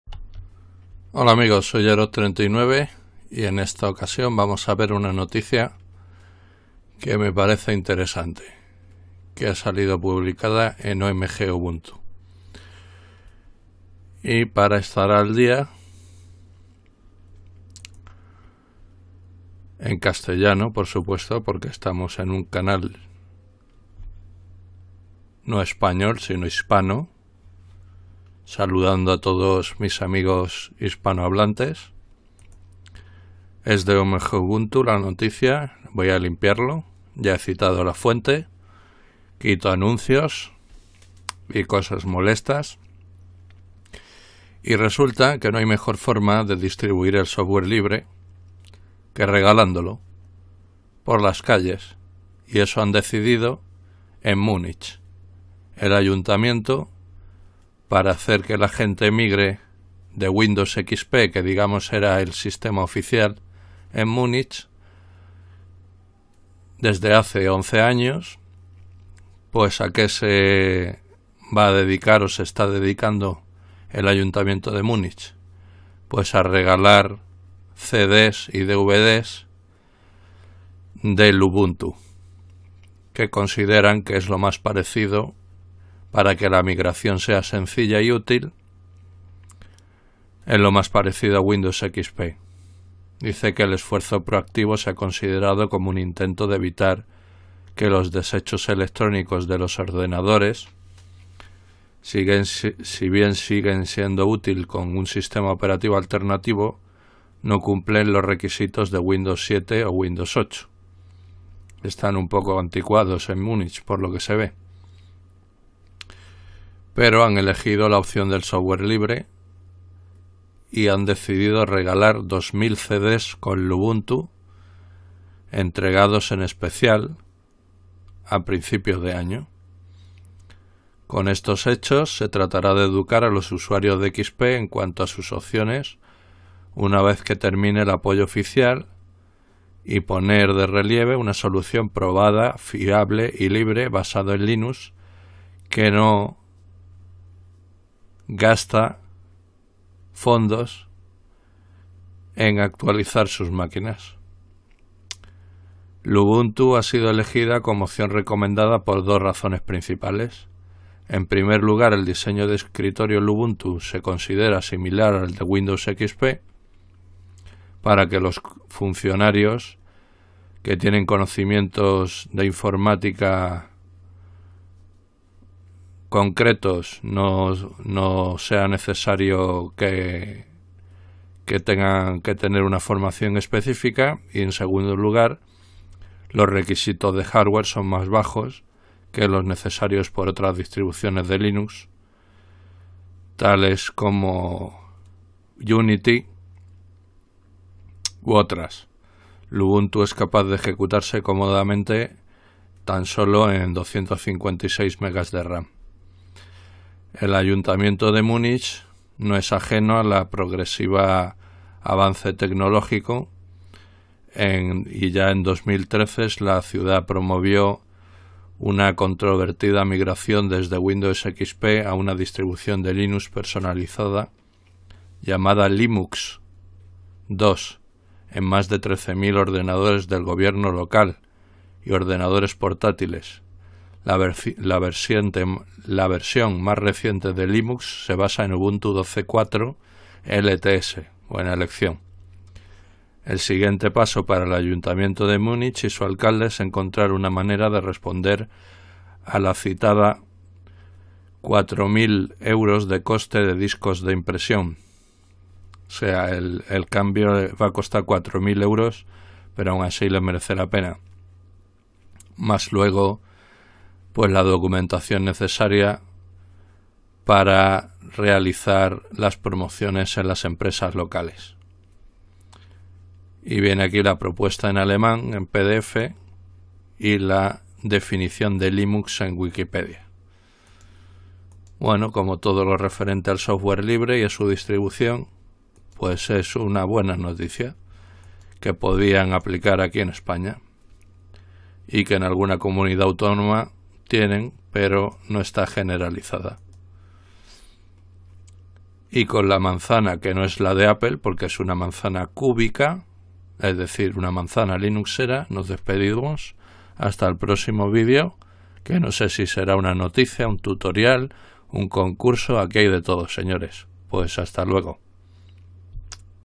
TODO UBUNTU NOTICIAS
EN MUNICH VAN A REGALAR MILES DE CD'S LINUXEROS PARA QUE LA GENTE ABANDONE WINDOWS : UBUNTU LINUX